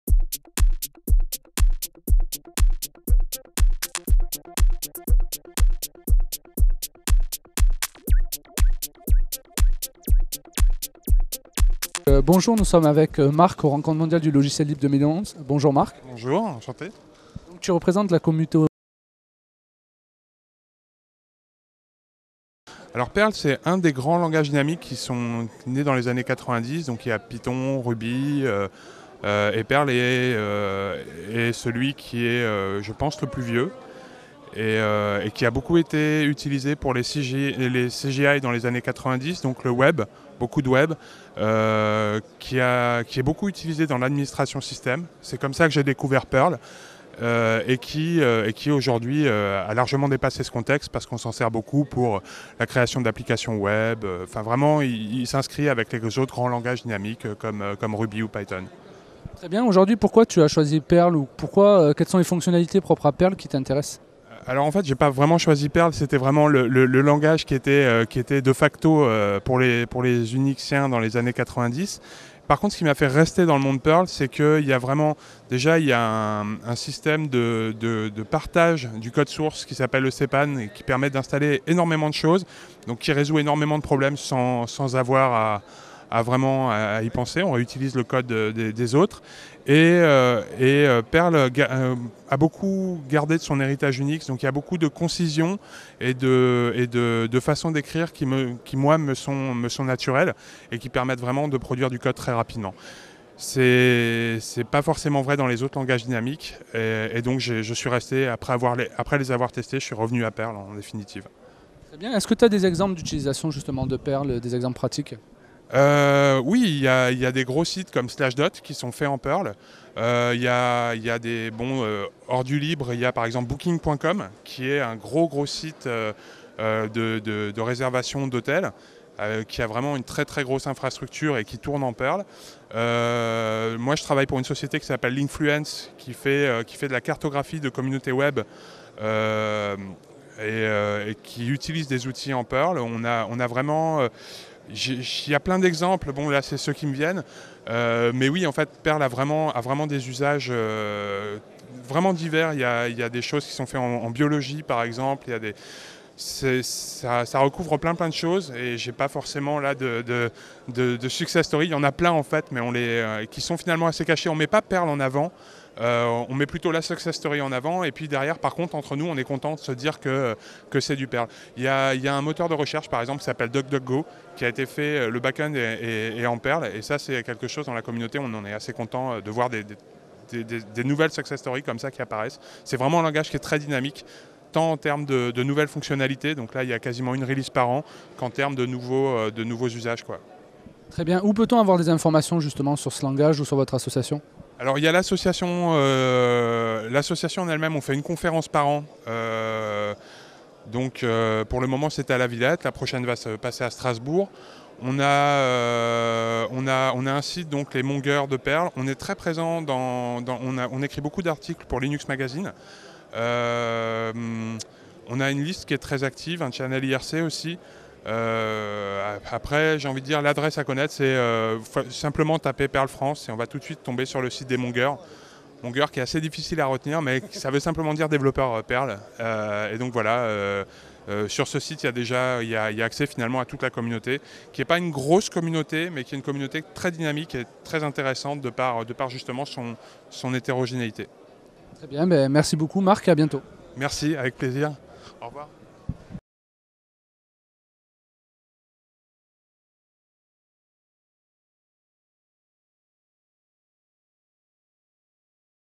Qu'est-ce que Perl ? langage de programmation dynamique open source
Marc Chantreux , Perl Mongers, nous parle du langage de programmation open source Perl  aux RMLL 2011. Il est interviewé par Jonathan Le Lous , Intelli'N TV.
hhtp://pm.org/